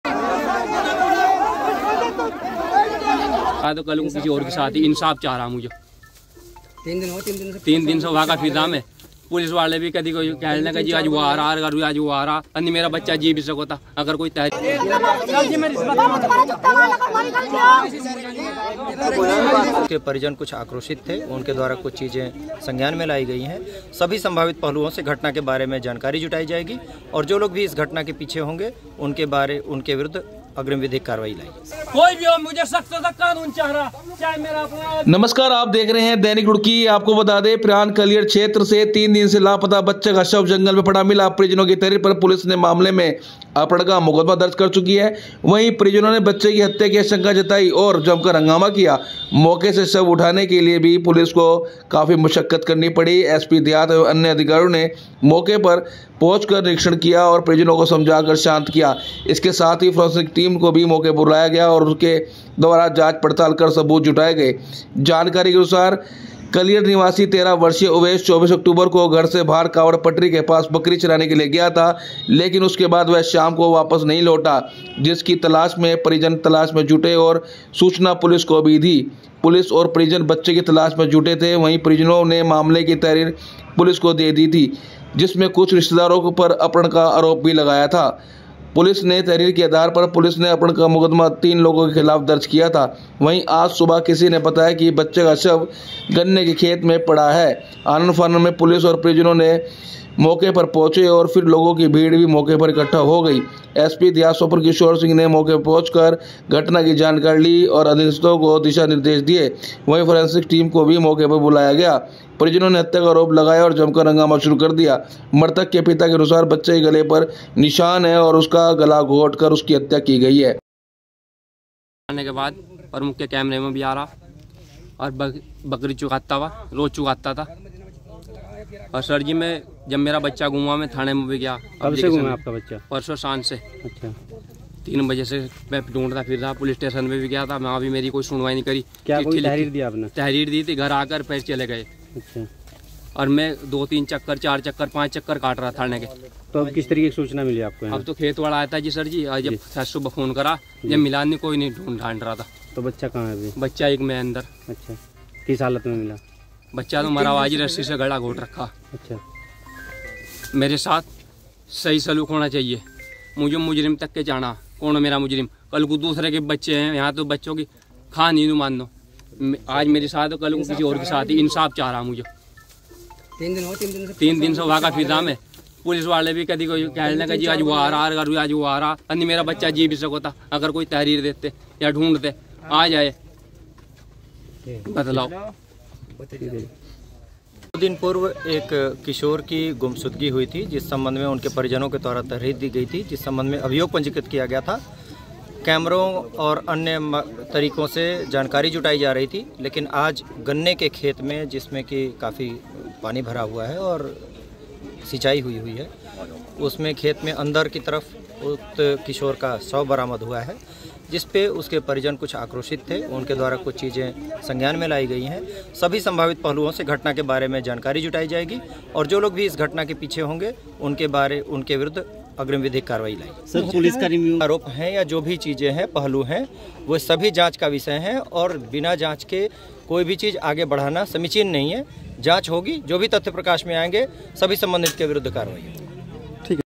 लापता बच्चे का शव गन्ने के खेत में पड़ा मिला-परिजनों ने हत्या का आरोप लगाते हुए किया हंगामा
तीन दिन से लापता बच्चे का शव गन्ने के खेत में पड़ा मिला-परिजनों ने हत्या का आरोप लगाते हुए किया हंगामा.